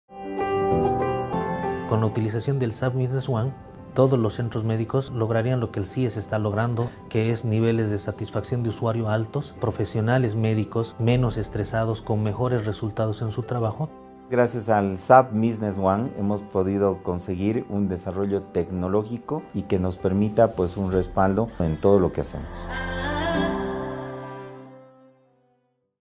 CIES Bolivia achieves reproducible success with SAP Business One [Short version]
Centro de Investigacion, Educacion y Servicios (CIES) is an non-profit that provides sexual and reproductive health services to 102,000 patients annually, through a network of 17 treatment centers across Bolivia. SAP Business One has given the NGO the tools to expand its reach while reducing stress on medical professionals and improving overall performance, providing a blueprint for success to other medical centers.